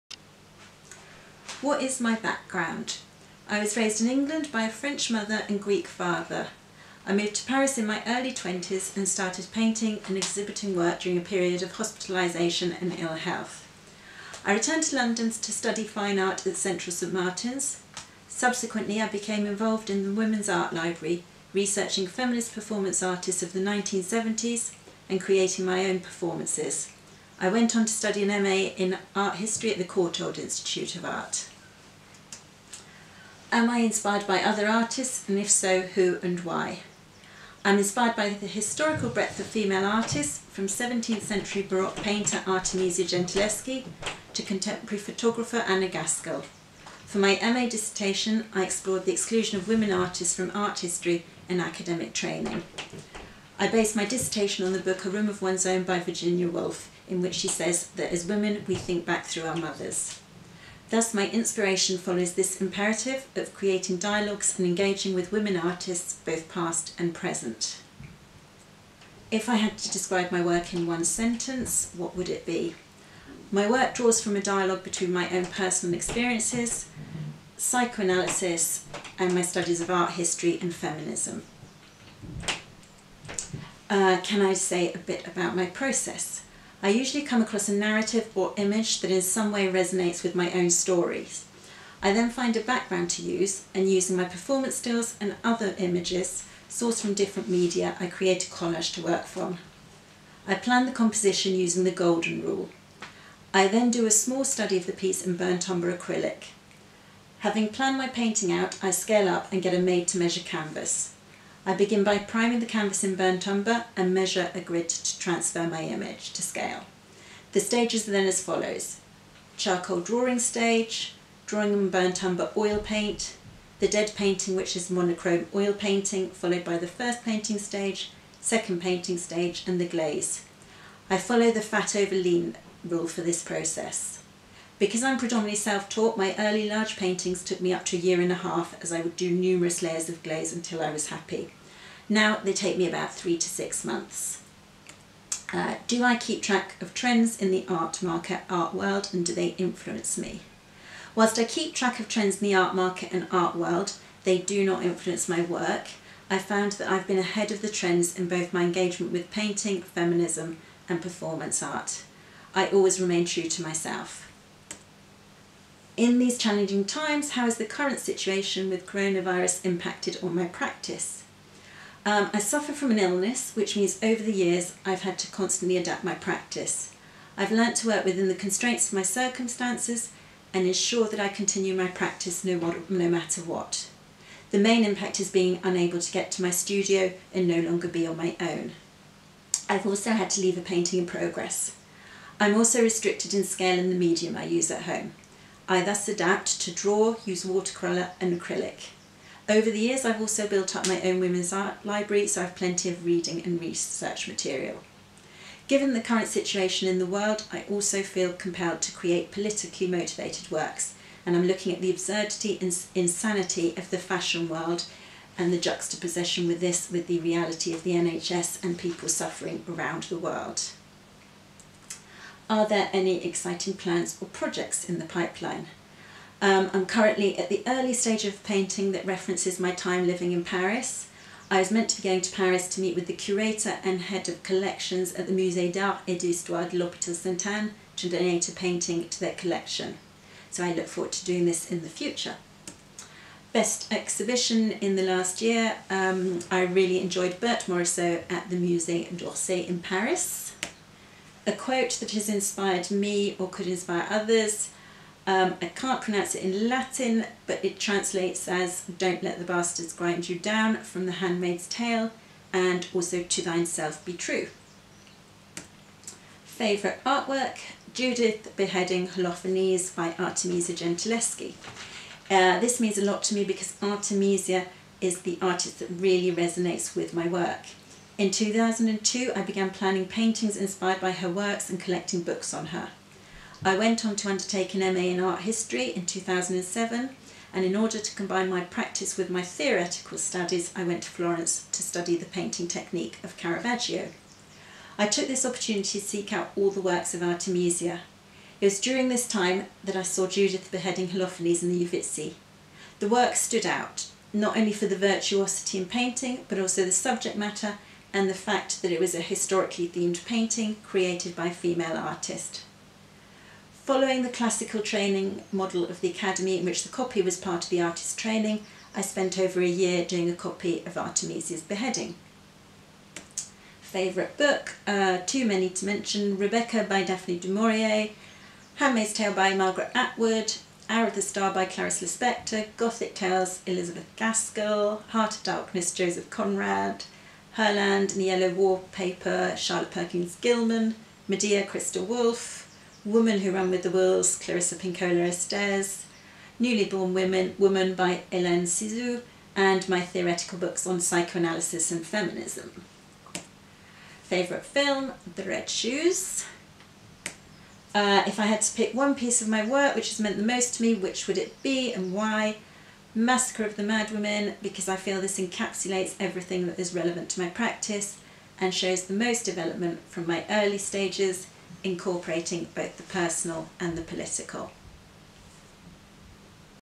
Interview with Artist Natalie Papamichael
An Interview to find out more about Artist Natalie Papamichael

Natalie tells us about here background and where it all started for her. She discuses her process and the techniques she uses to create her artworks. Natalie also talks about the influences in her works.